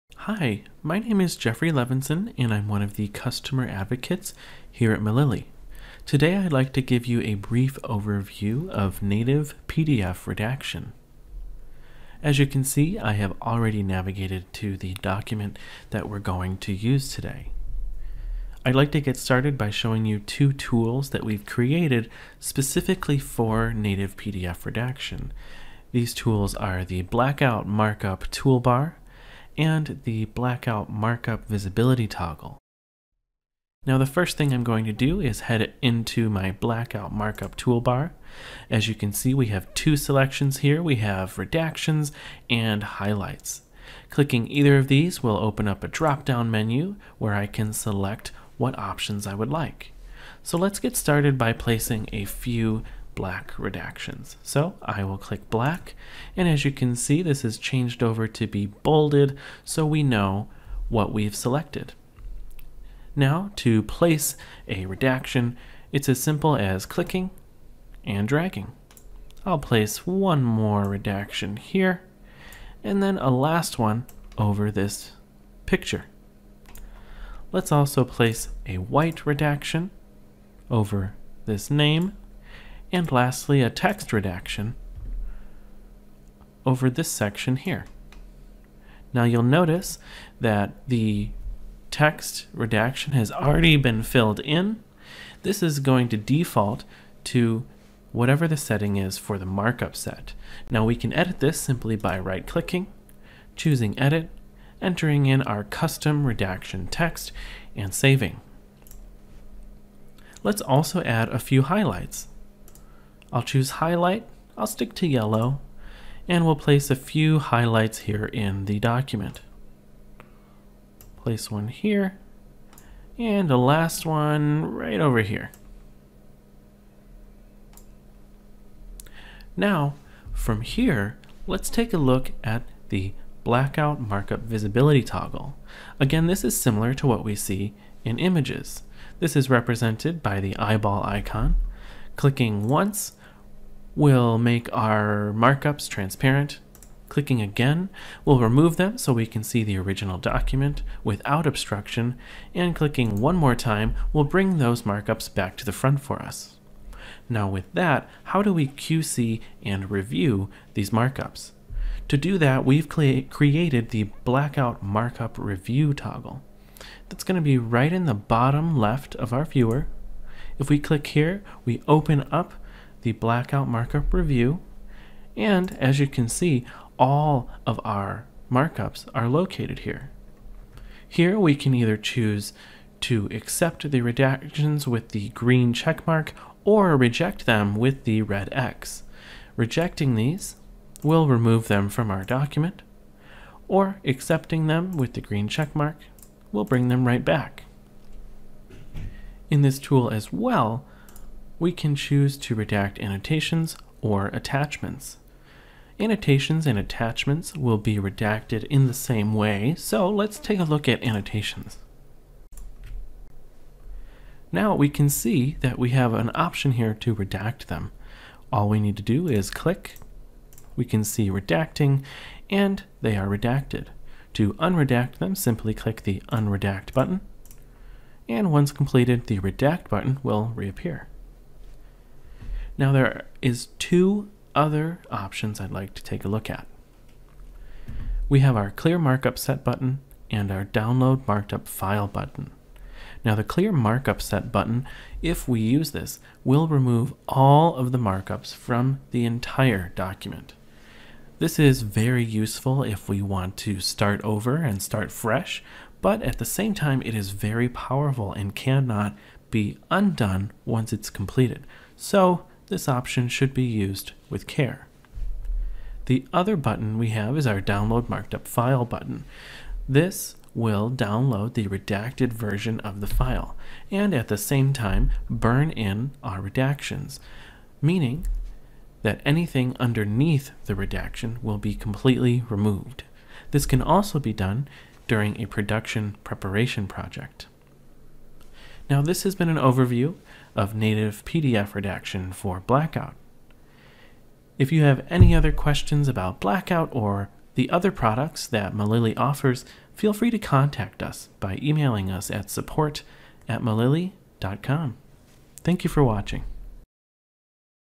Demonstrating Blackout's Native PDF Markups
This video demonstrates how to place native PDF markups with Blackout for Relativity.